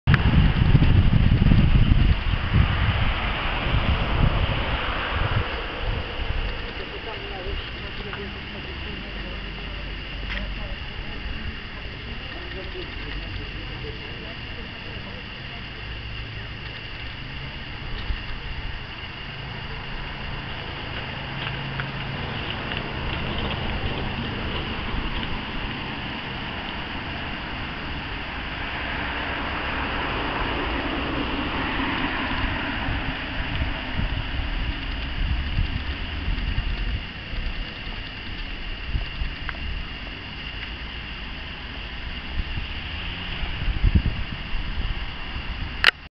Jaworzno Moje Miasto 29.09.2011 Test Fuji S2950 cz2
Dziś zaczynam wycieczkę Od Jaworzna Do Krakowa przez:
Chrzanów,Trzebinia(możliwe),Krzeszowice